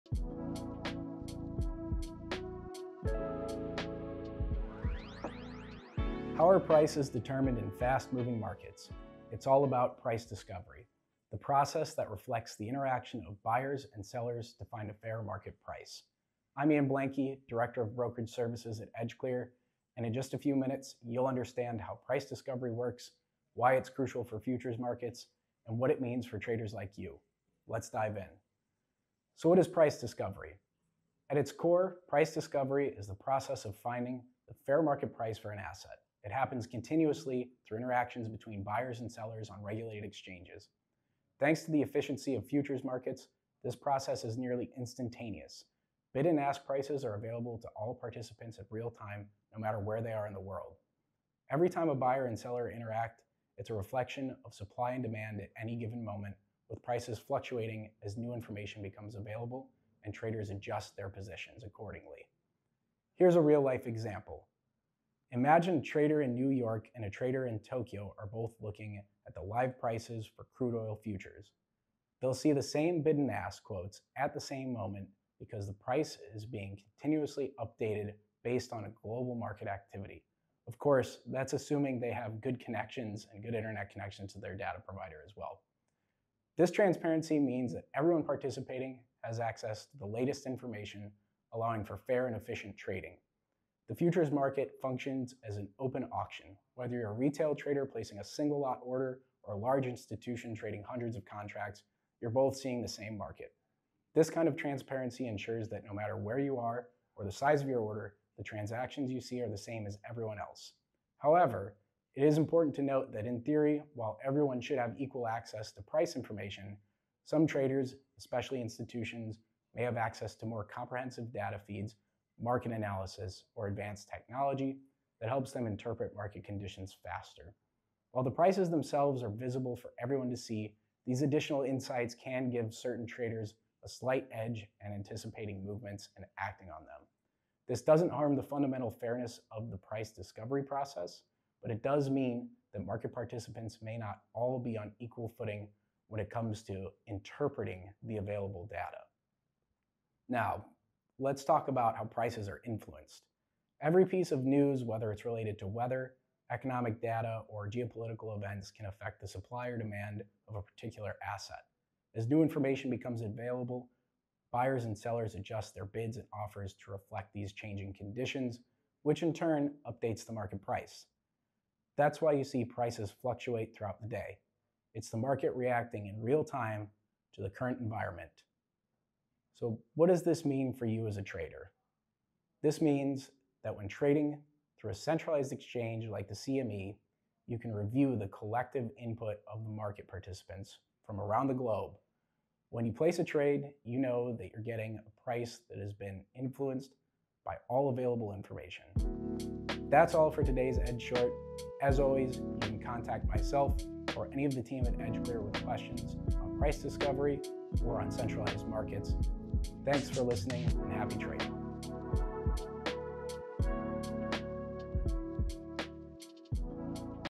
Futures Market Price Discovery Explained
How does price discovery impact futures trading? Join EdgeClear's Director of Brokerage Services, Ian Blanke, to explore this critical market mechanism and how it helps traders make informed decisions.

0:00 Intro
0:29 What is price discovery?
1:05 Real-life Example
2:30 How are prices influenced?
3:00 What does this mean for traders?
3:19 Outro - Thanks for watching!